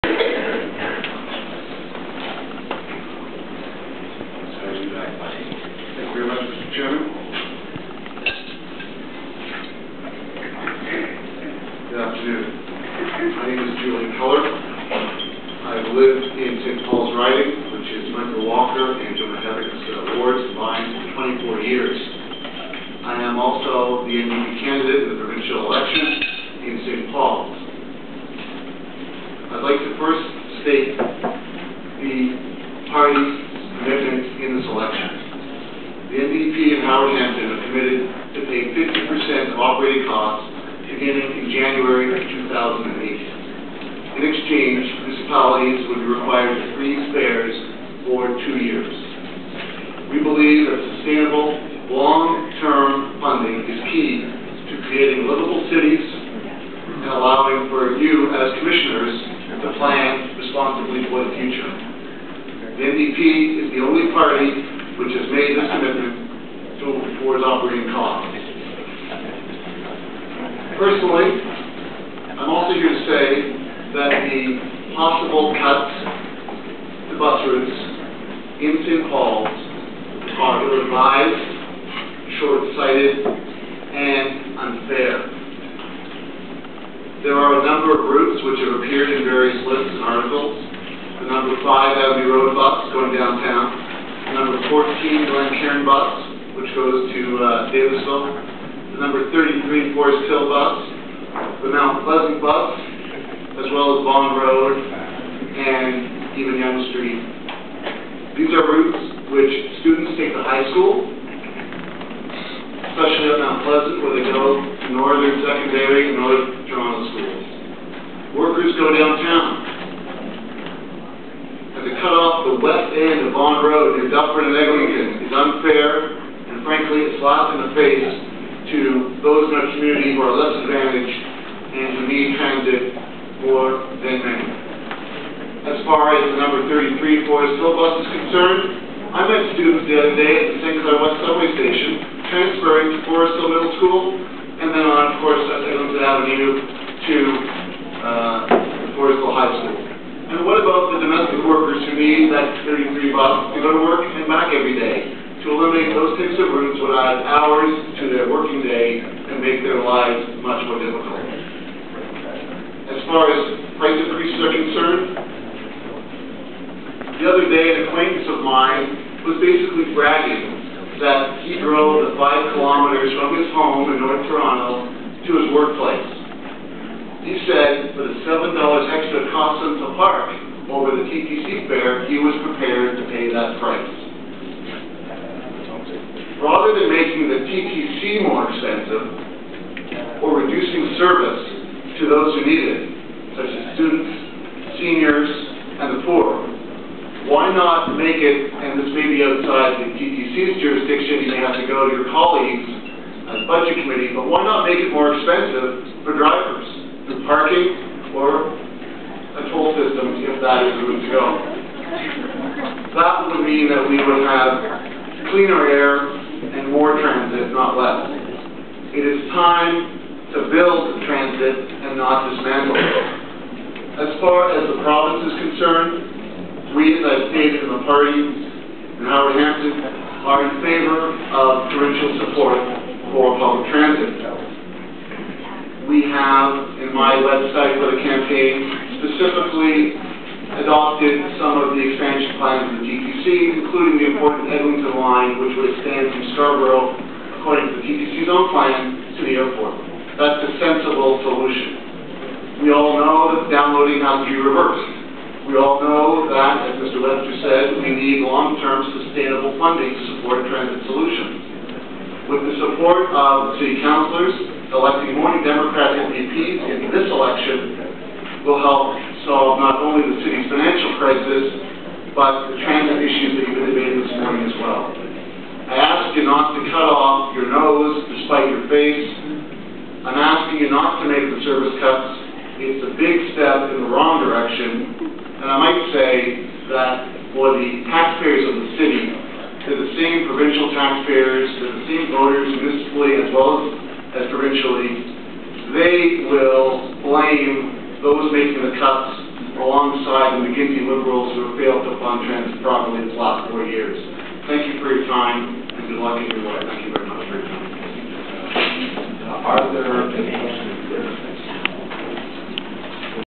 Public Consultation and vote on TTC Service Cuts, Fare Hike
Public Consultation on TTC Service Cuts or Fare Hike

Committee Room 1
Toronto City Hall